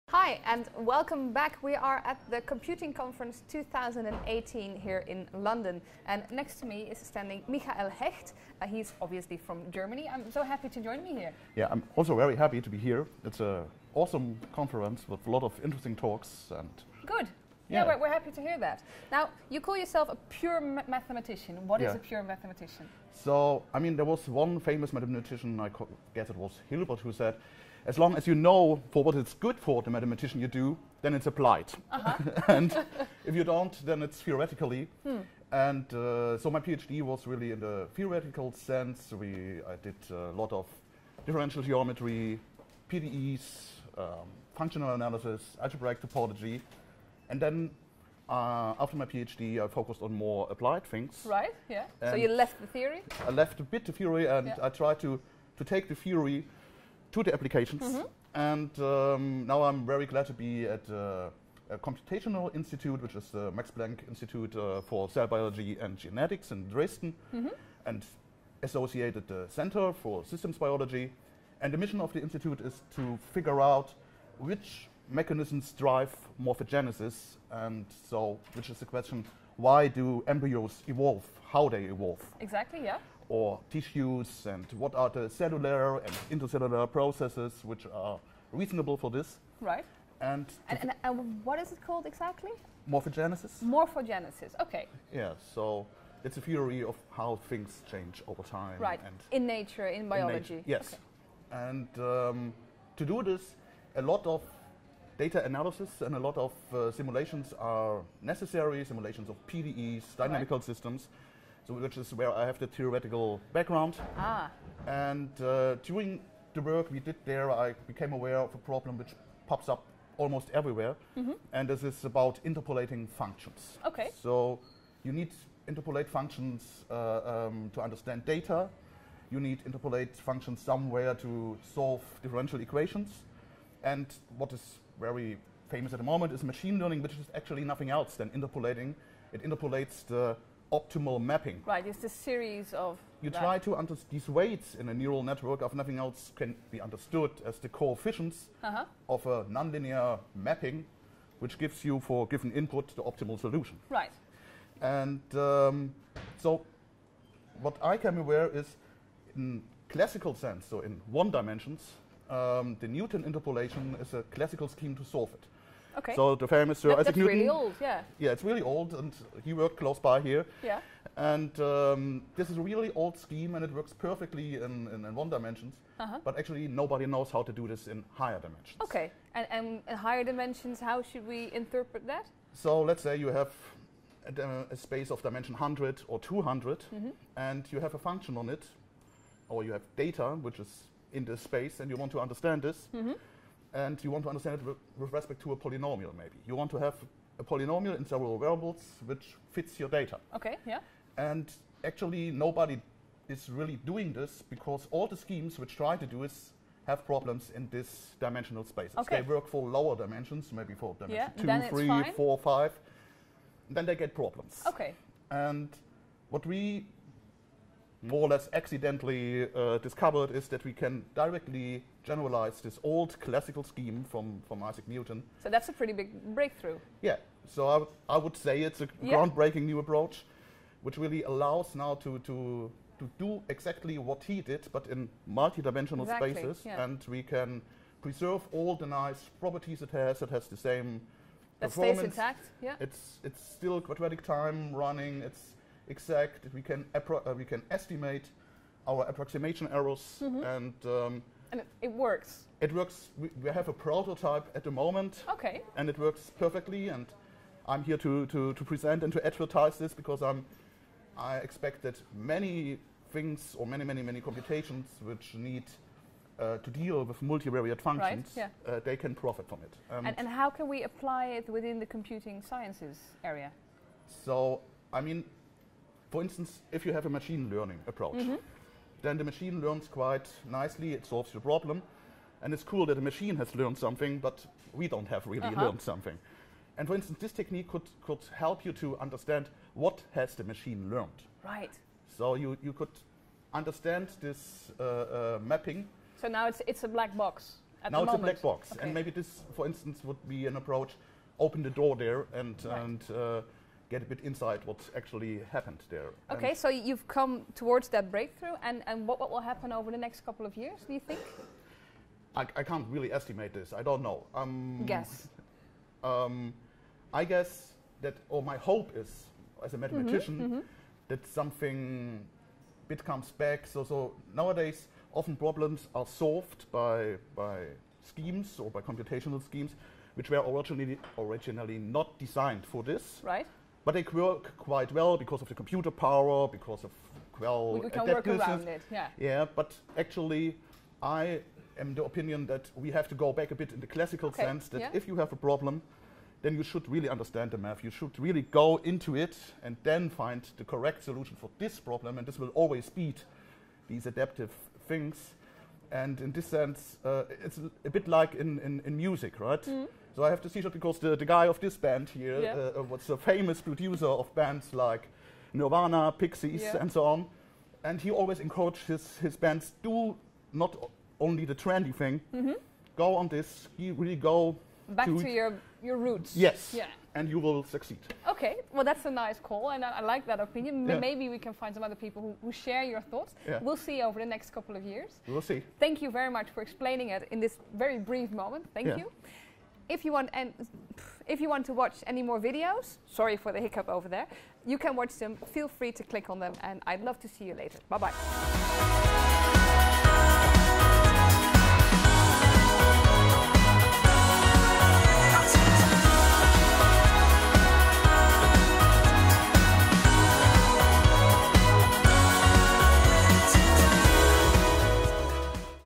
Interview with Michael Hecht from Max Planck Institute of Molecular Cell Biology and Genetics
For me, as a pure mathematician, my aim is to use and further develop deep mathematical results in a way that allows to realize new fundamental computational schemes. For instance, by using concepts of algebraic geometry I could generalize the Newton interpolation scheme from its classical 1–dimensional case to arbitrary dimensions without loosing any of its excellent approximation abilities or performance properties. Especially, in high dimensions I want to apply this ground-breaking approach to concepts like computation of quadrature’s, non-linear optimization, linear and polynomial regression, spectral particle methods for ODE & PDE Solvers, analysis of deep neural networks, data compressing, filtering and dimension reduction.
Other interest are concerning morphogenetic processes in biology. Thereby the computation and analysis of topological invariants of dynamical systems and algebraic topology with focus in graph theory play a crucial role.

Abstract
In scientific computing, the problem of finding an analytical representation of a given function   f:Ω⊆Rm⟶R,C  is ubiquitous. The most practically relevant representations are polynomial interpolation and Fourier series. In this article, we address both problems in high-dimensional spaces. First, we propose a quadratic-time solution of the Multivariate Polynomial Interpolation Problem (PIP), i.e., the N(m, n) coefficients of a polynomial Q, with   deg(Q)≤n , uniquely fitting f on a determined set of generic nodes   P⊆Rm  are computed in   O(N(m,n)2)  time requiring storage in   O(mN(m,n)) . Then, we formulate an algorithm for determining the N(m, n) Fourier coefficients with positive frequency of the Fourier series of f up to order n in the same amount of computational time and storage. Especially in high dimensions, this provides a fast Fourier interpolation, outperforming modern Fast Fourier Transform methods. We expect that these fast and scalable solutions of the polynomial and Fourier interpolation problems in high-dimensional spaces are going to influence modern computing techniques occurring in Big Data and Data Mining, Deep Learning, Image and Signal Analysis, Cryptography, and Non-linear Optimization.